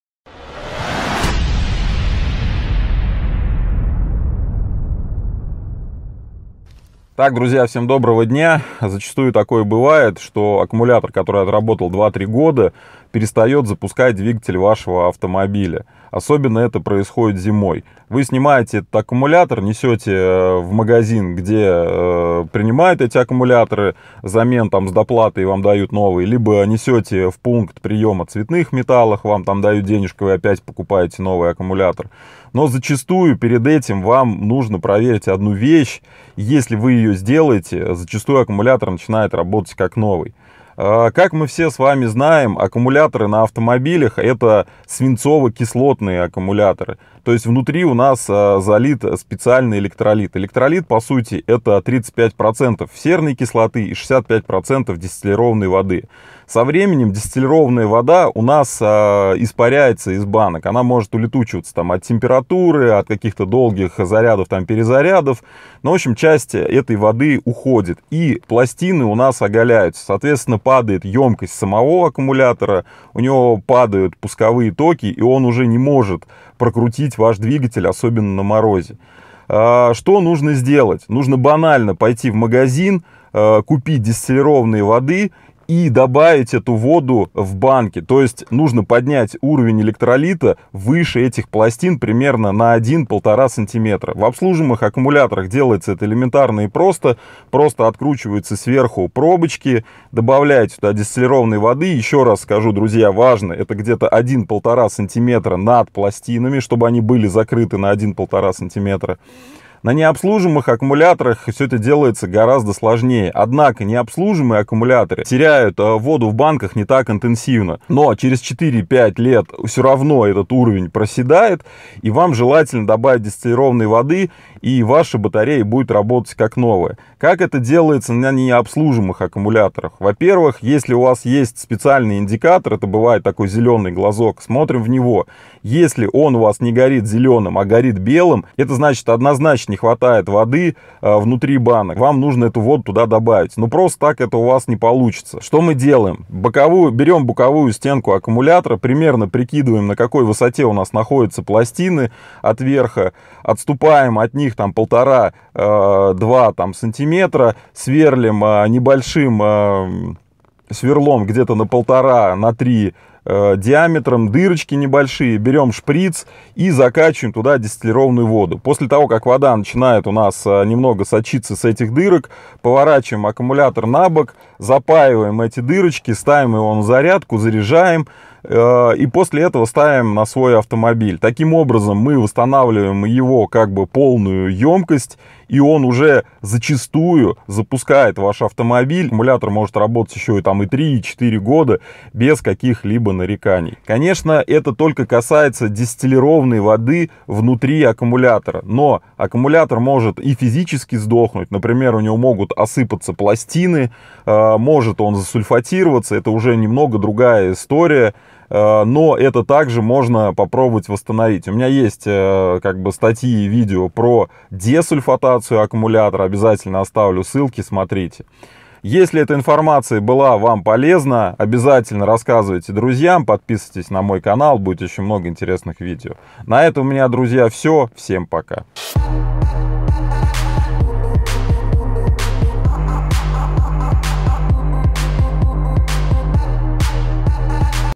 НЕ ПОКУПАЙ НОВЫЙ АККУМУЛЯТОР! ПОКА НЕ ПРОВЕРИШЬ ...
Перед покупкой нового аккумулятора сначала проверь в своем старом это. Возможно причина его не работоспособности элементарна и банальна ...